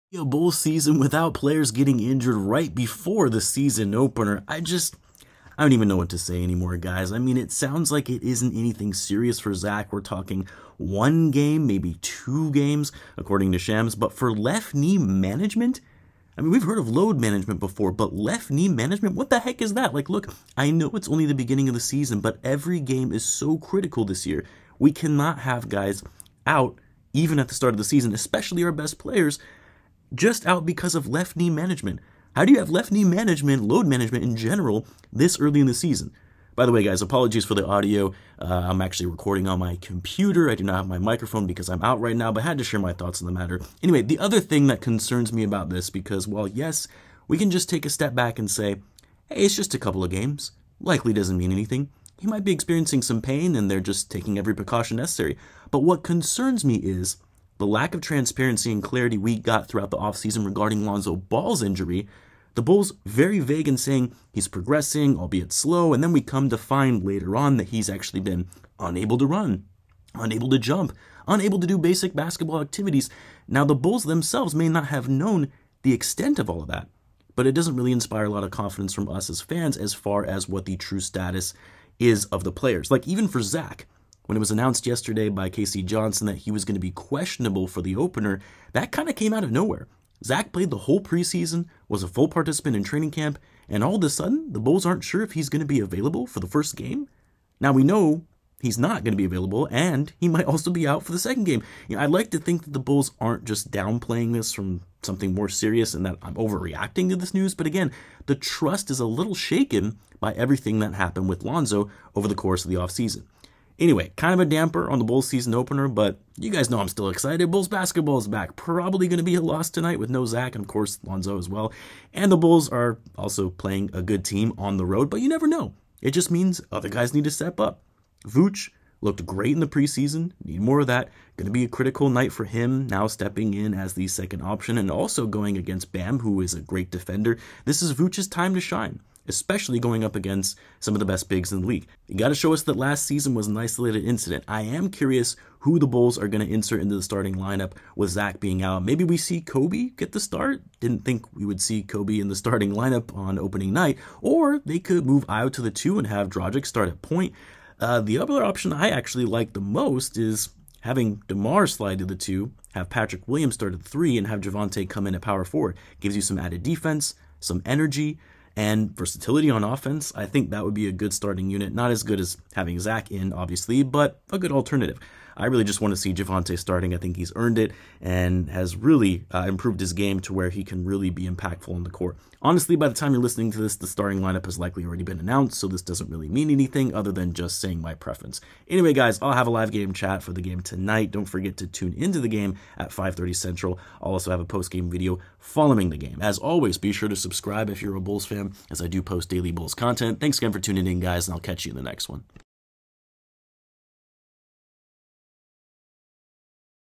Zach Lavine OUT For Bulls Season Opener….Great
Sharing thoughts on the news regarding Zach Lavine missing the Bulls season opener